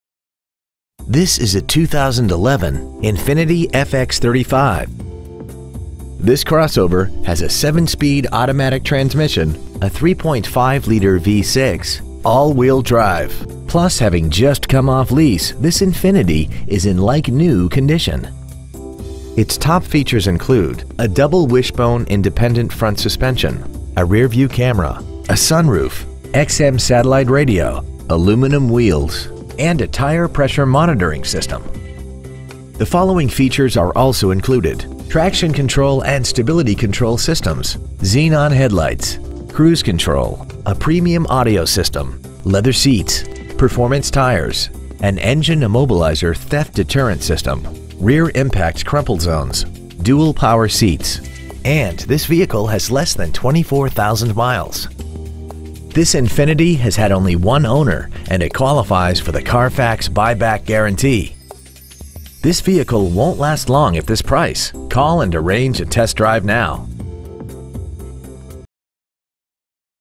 Preowned 2011 Infiniti FX35 Coconut Creek FL
Year : 2011

 Make : Infiniti

 Model : FX35

 Engine : Gas V6 3.5L/213

 Trans . : Automatic

 Exterior : Malbec Black

 Miles : 23,228

 Interior : Graphite

 Stock : 35627

 Offleaseonly.com

 3531 Lake Worth Rd

 Preowned 2011 Infiniti FX35 Lake Worth FL